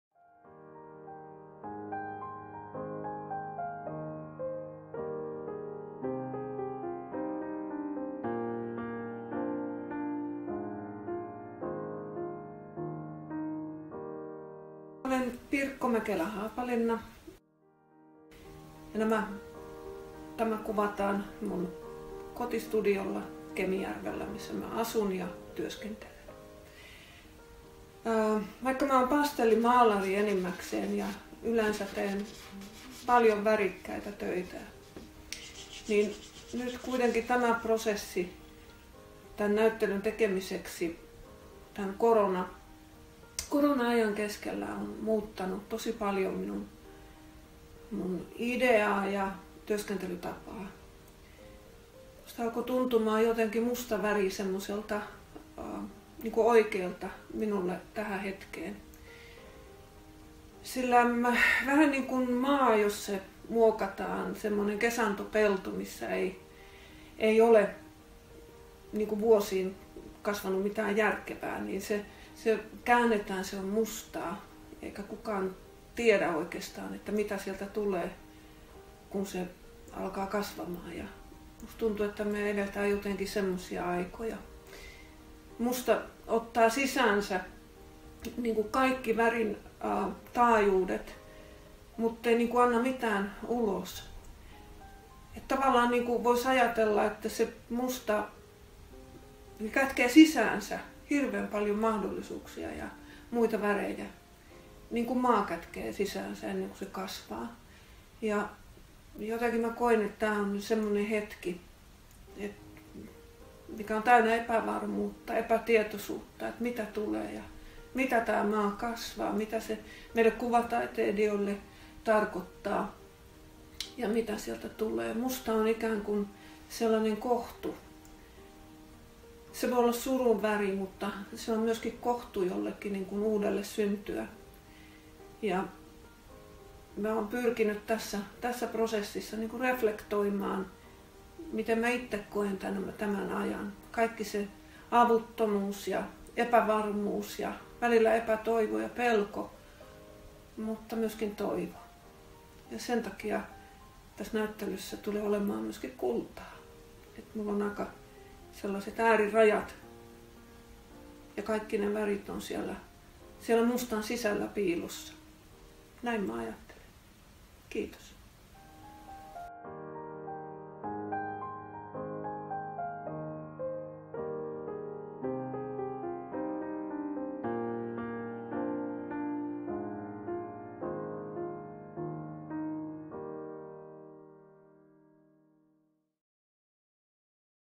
Musta Tuntuu - Pirkko Mäkelä-Haapalinna
***Please scroll down for the english version***
Lapin taiteilijaseuran videolla taiteilija Pirkko Mäkelä-Haapalinna kuvailee uuden Musta tuntuu-näyttelynsä luovaa prosessia. Näyttelyn teokset tehtiin Covid-19-pandemian aikana ja videolla Pirkko Mäkelä-Haapalinna kuvailee lähestymistapaansa, metodejaan ja ajatteluaan tämän kompleksisen tilanteen käsittelyssä.
Artists' Association of Lapland presents a video describing the creative process behind the new exhibition Musta Tuntuu (I feel / Black feelings in Finnish) by artist Pirkko Mäkelä-Haapalinna. Artworks were made during pandemic of Covid-19 and Pirkko Mäkelä-Haapalinna explains her approach, methods, way of thinking, dealing and reflecting this complicated situation.

Lapin taiteilijaseura/Artists' Association of Lapland
Video: Miroslava Mayerová
Subtitles: Iina Miettunen
2020